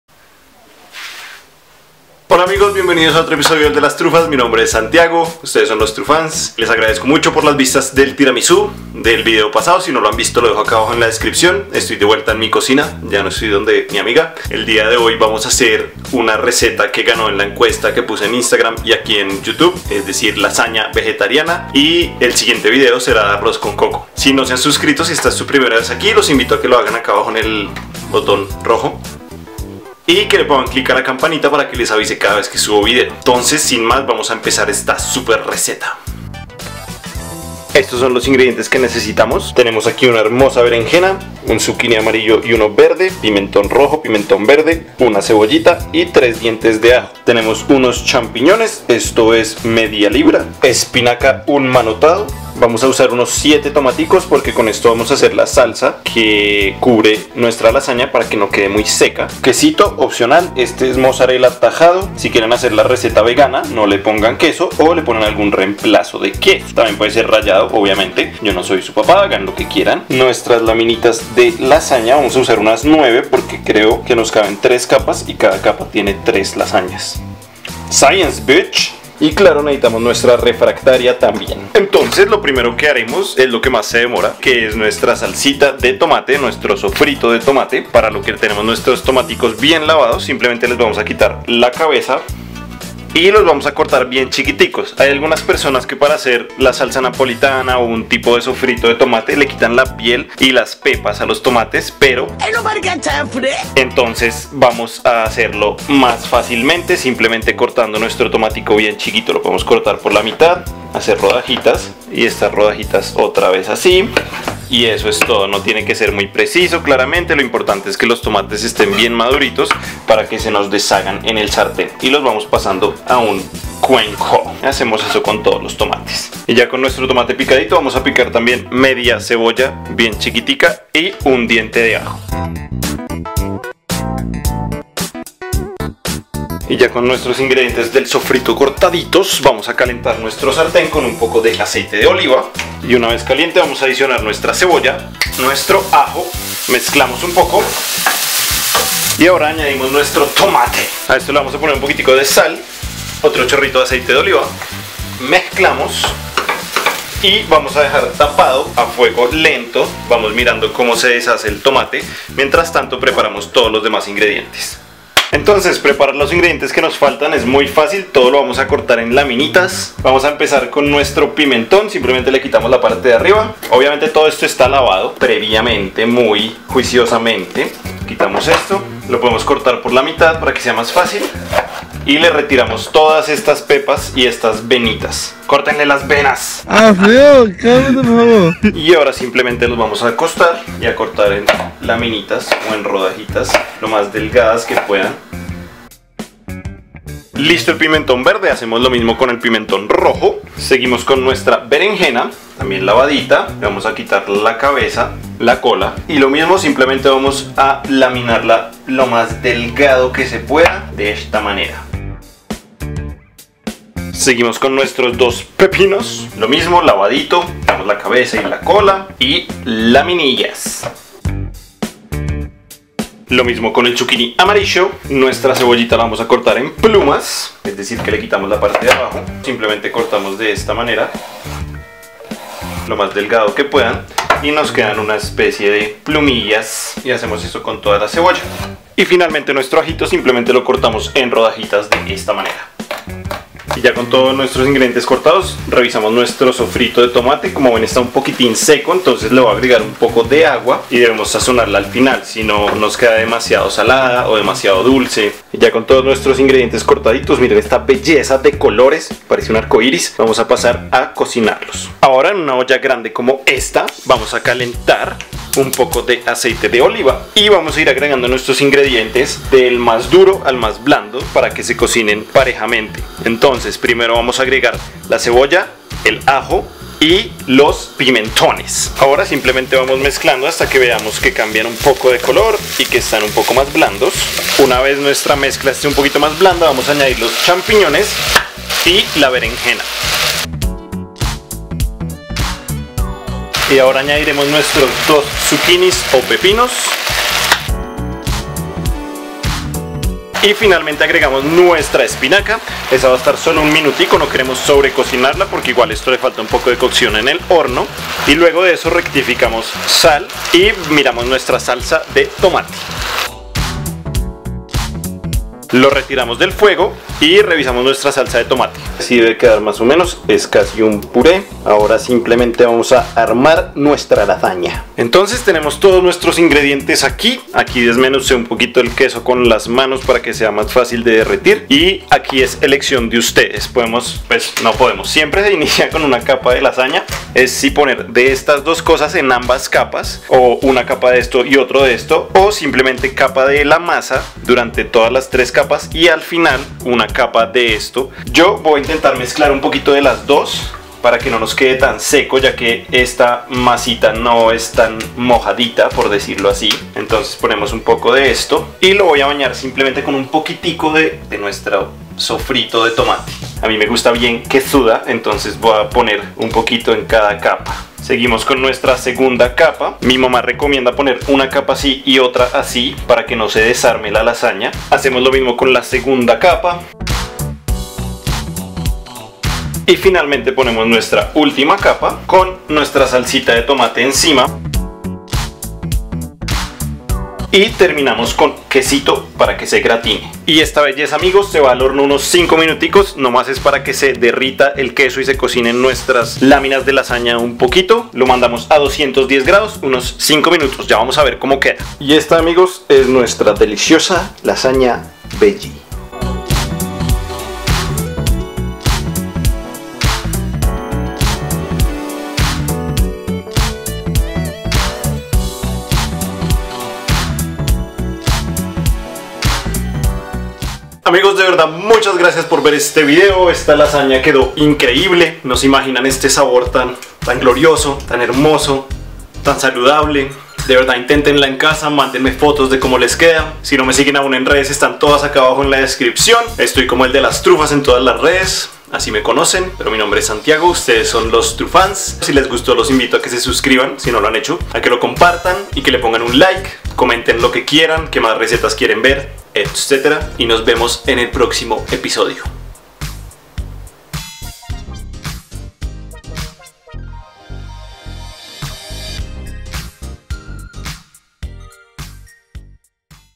Lasaña Vegetariana | El de las trufas
¡En este video les enseño una receta super fácil y rica para hacer lasaña vegetariana!

Ingredientes
1 Bereneja
1 Calabacín Verde
1 Calabacín Amarillo
1 Pimentón Rojo
1 Pimentón Verde
1 Cebolla
3 Dientes de Ajo
250 gr de Champiñones
300 gr Espinaca
7 Tomates
250 gr Queso Mozarella
Láminas de Lasaña